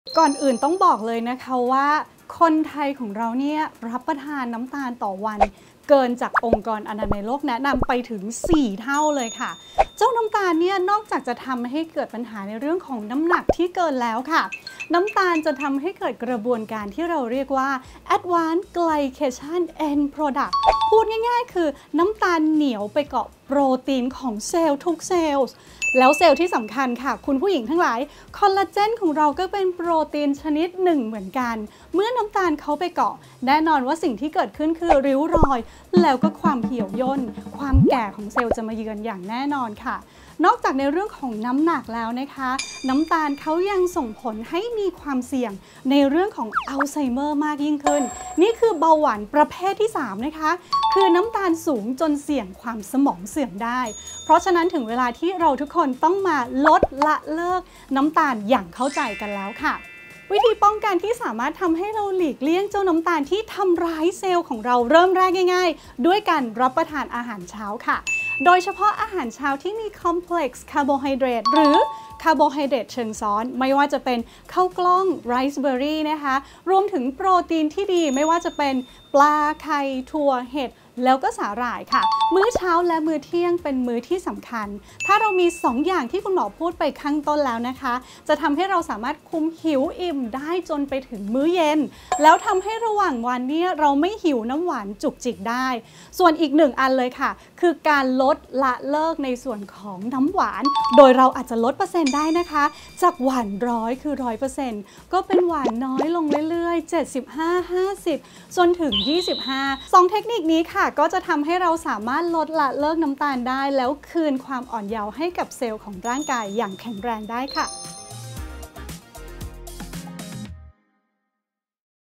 ลด ละ เลิก การทานน้ำตาลอย่างเข้าใจ คืนความอ่อนเยาว์ให้กับร่างกาย
การทาน น้ำตาล มากเกินไป นอกจากทำให้น้ำหนักขึ้นแล้วยังทำให้เกิดริ้วรอยบนใบหน้าอีกด้วย และยังเสี่ยงเป็นโรคอัลไซเมอร์ จะมีวิธีป้องกันและหลีกเลี่ยงอย่างไรบ้าง ไปฟังกันค่ะ
สามารถสอบถามเพิ่มเติมได้ที่